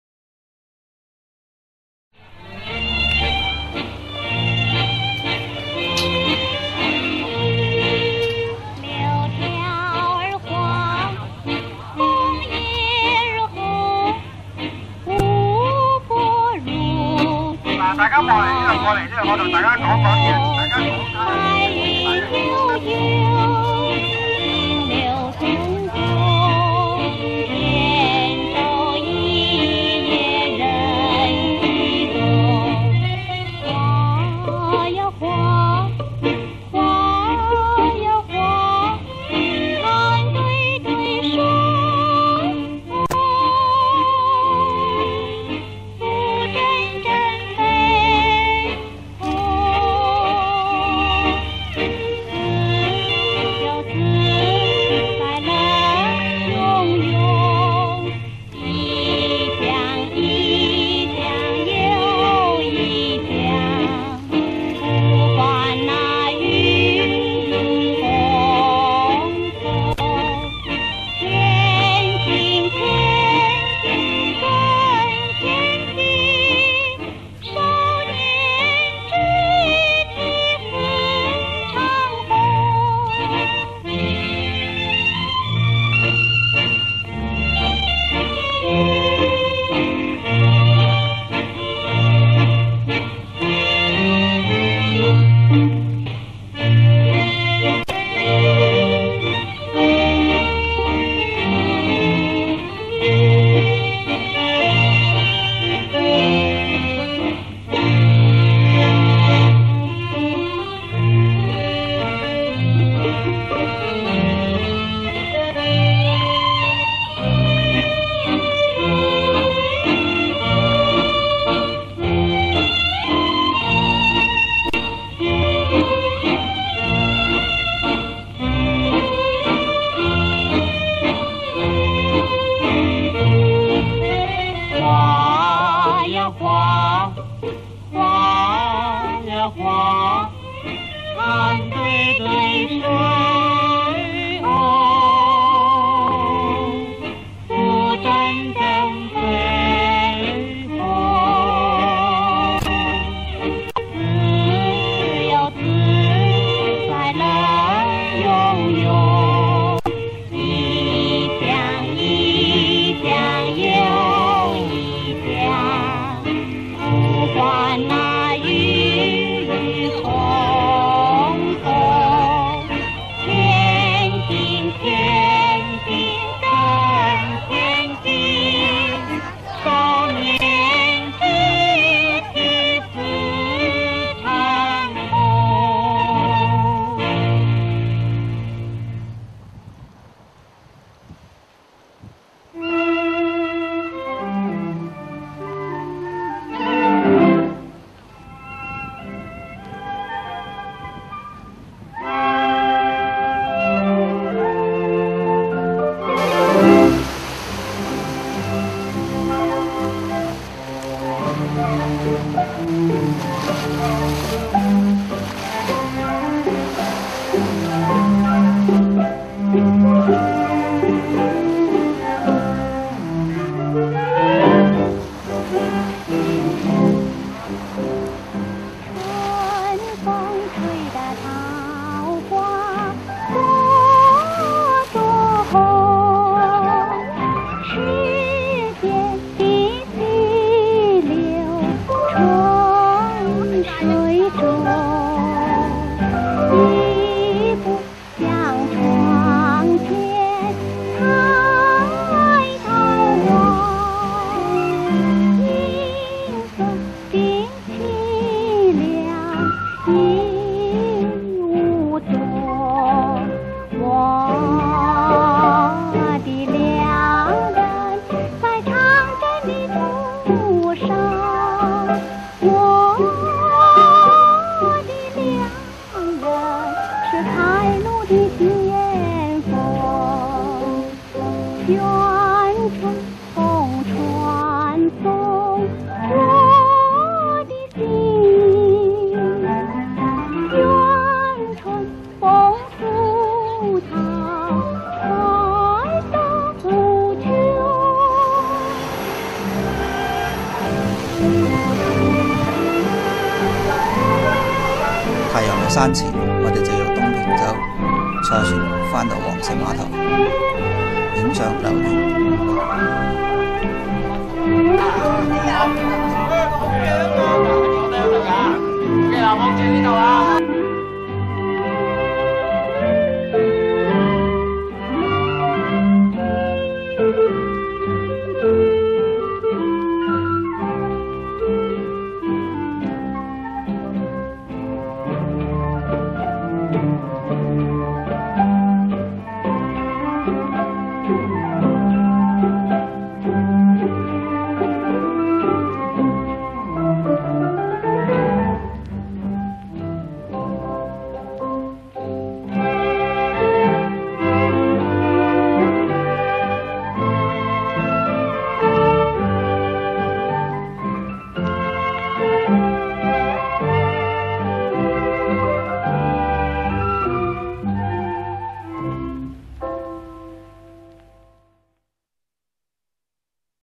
聽住老歌去旅行 香港東坪洲 (2008 11 16)
歌迷會2008年11月16日搭船遊覽了塔門及東坪洲